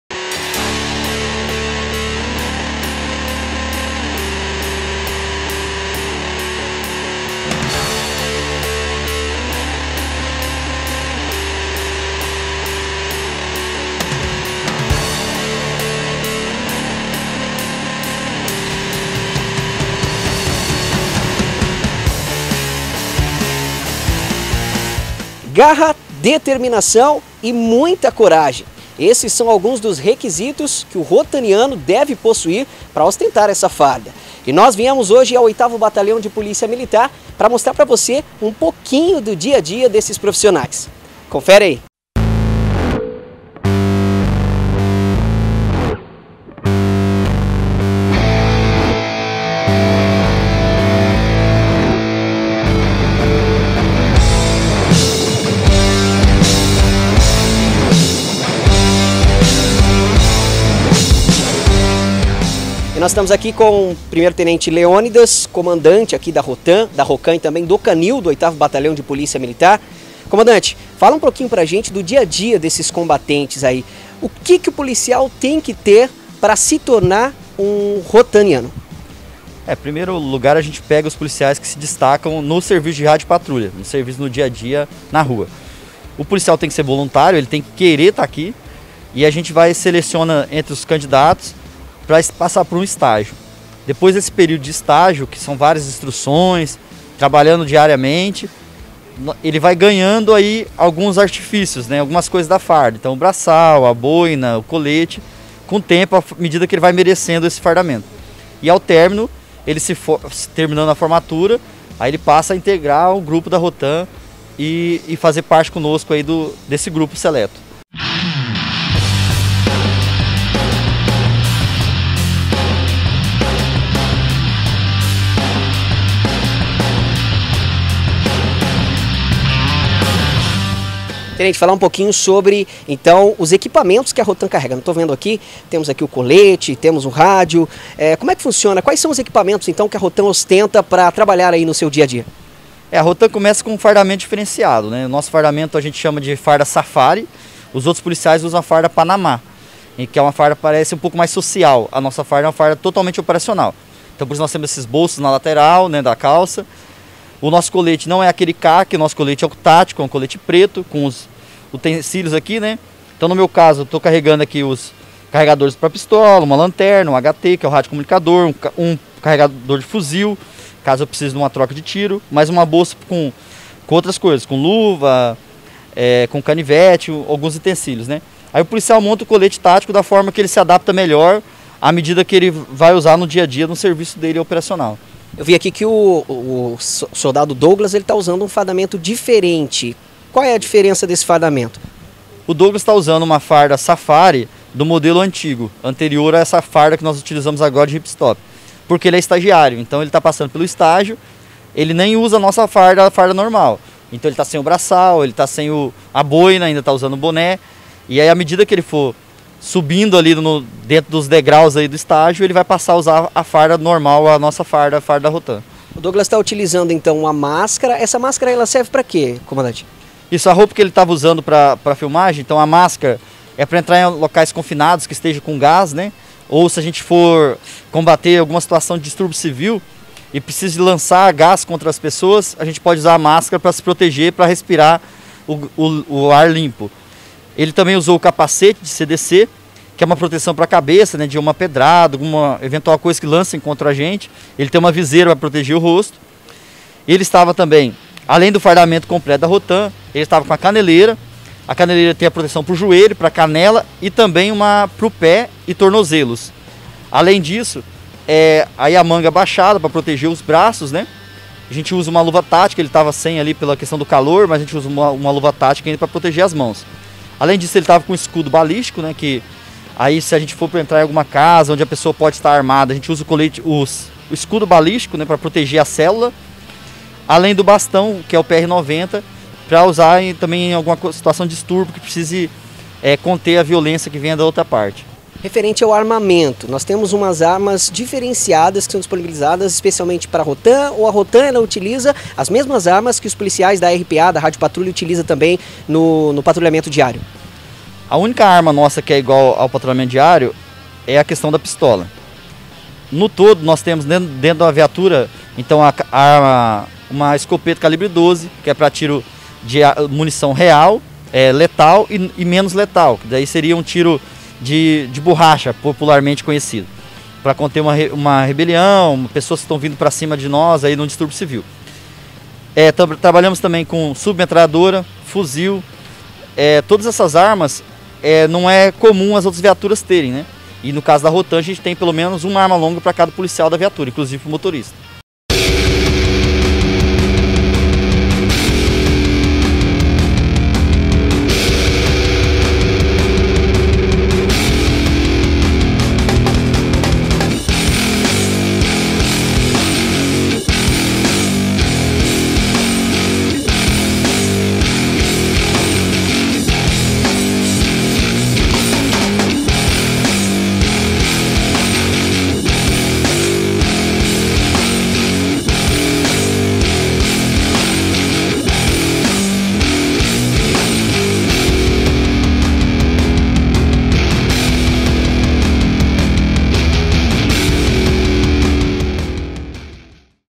Rotam 8ºBPM de Paranavaí
Acompanhe os trabalhos da ROTAM do 8ºBPM de Paranavaí na matéria do repórter Rodrigo lucas